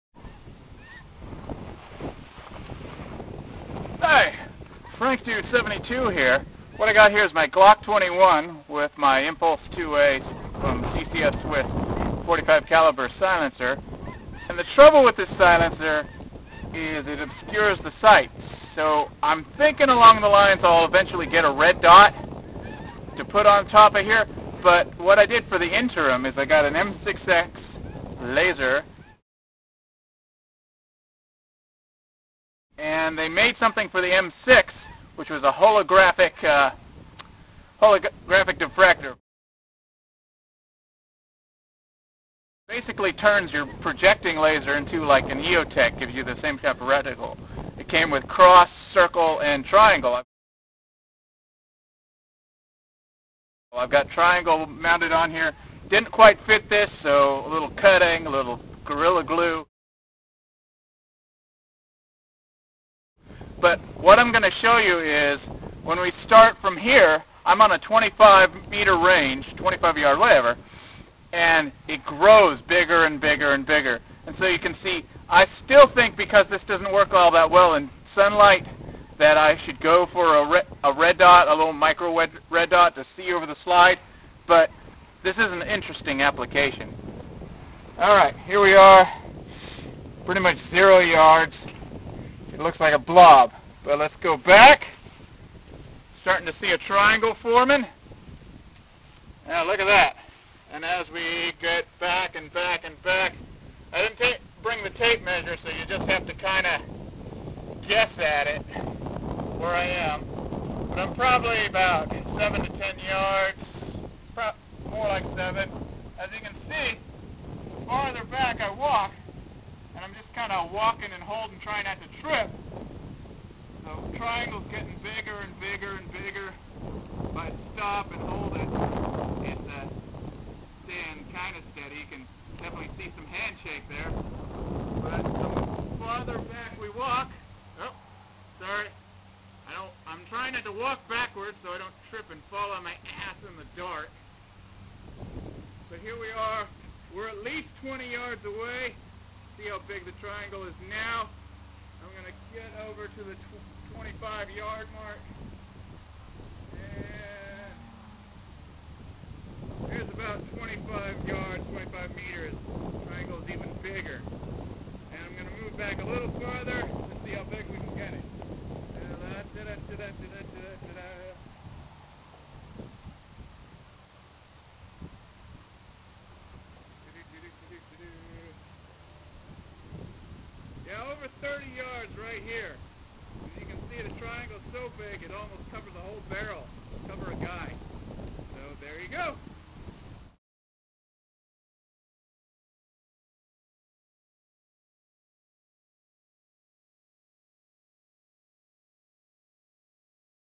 Predator Laser: Holographic Pattern Generator.
Ever wanted an EOTech's reticle pattern to project from your laser?

Well, back in 2002, Streamlight came out with a little accessory for the M6 weaponlight / laser called a Holographic Pattern Generator.

Me, I decided to be a rebel and tailor it to fit my Insight M6X.
So now I have a NVD compatible sight to use when my Glock 21 has a suppressor mounted. :D

This here's a quick demo of what the pattern looks like from 0-30+ yards.